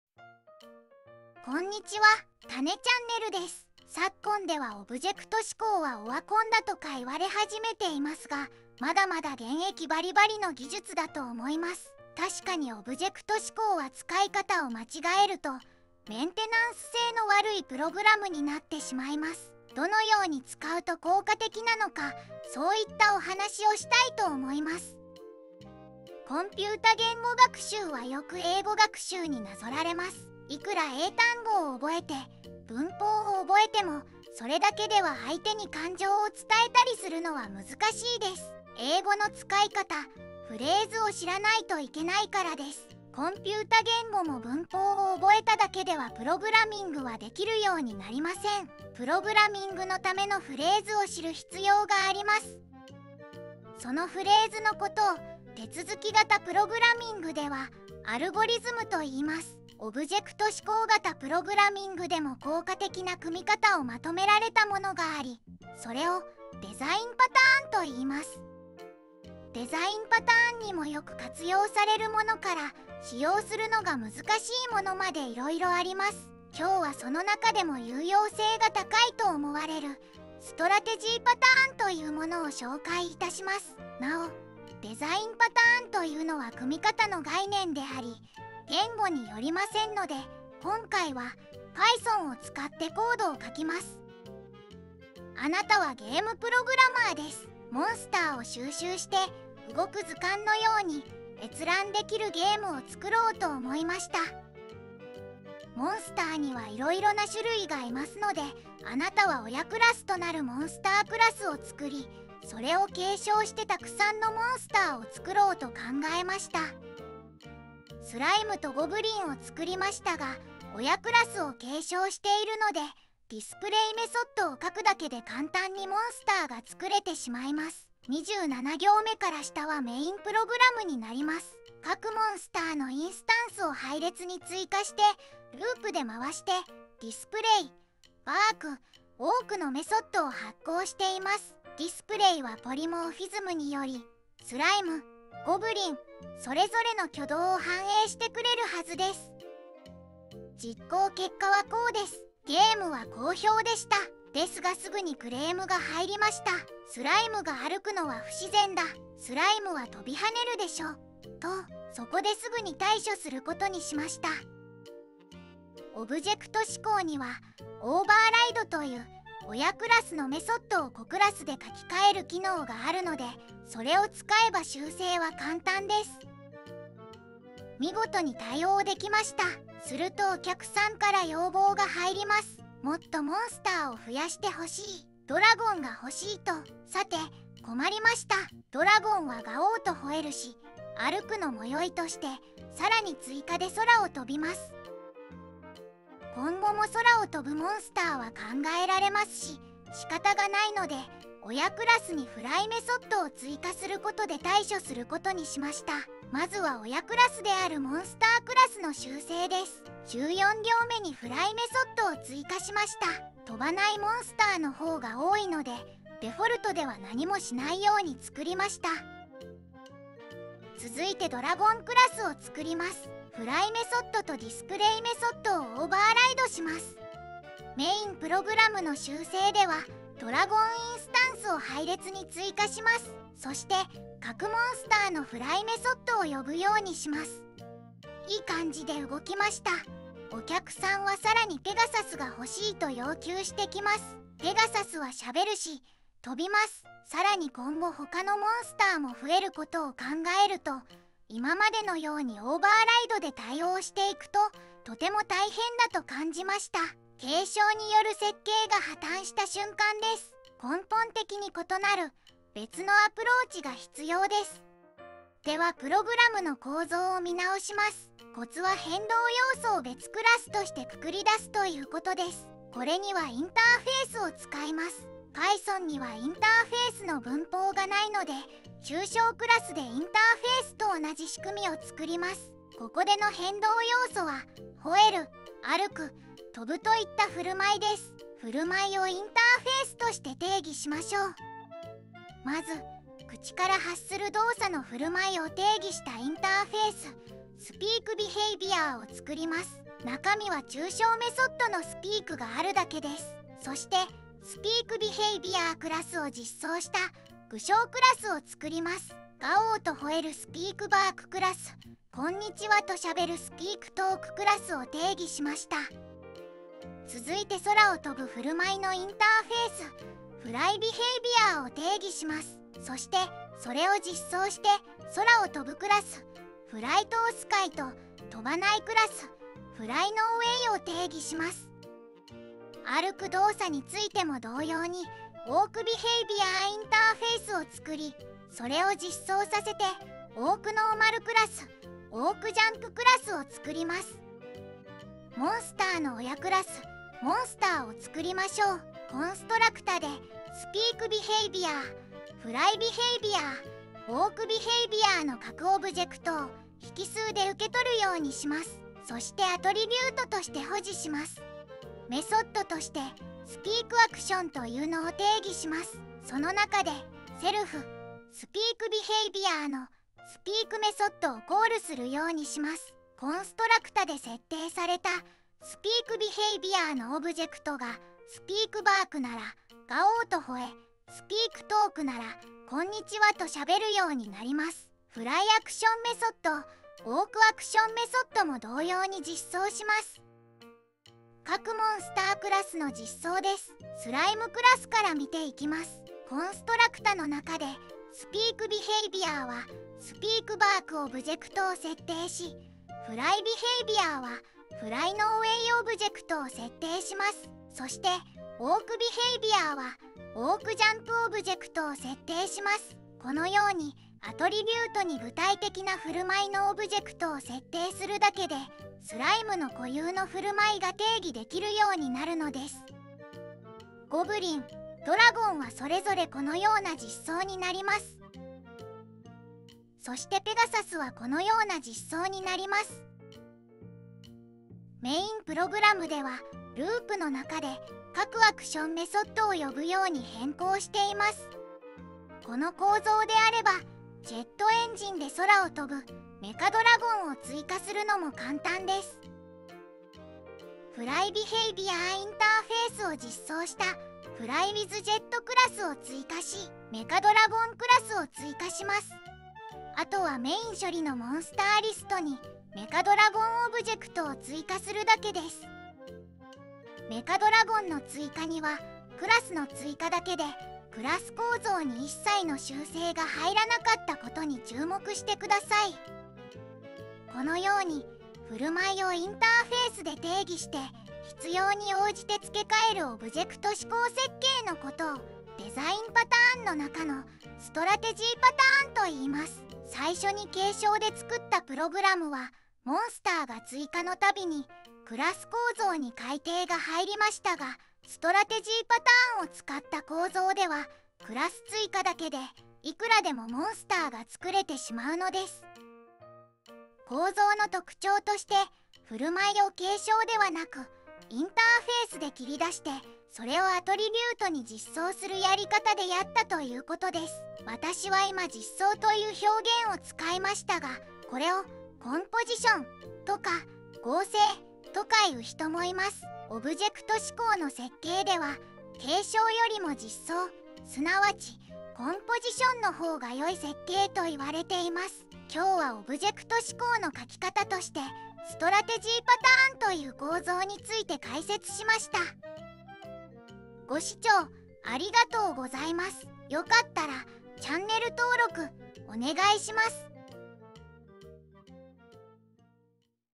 【オブジェクト指向の真髄】デザインパターン：Strategy Pattern の使い方を解説します【オブジェクト指向入門】
オブジェクト指向は、効果的な組み方をすることでその威力を発揮します。
そのお手本となる組み方はデザインパターンと言われています。
今回はその中で代表的なストラテジーパターンについて、どのように使うのかご紹介いたします。
なお、デザインパターンは言語に依らない概念ですので、Pythonを使用して解説を行います。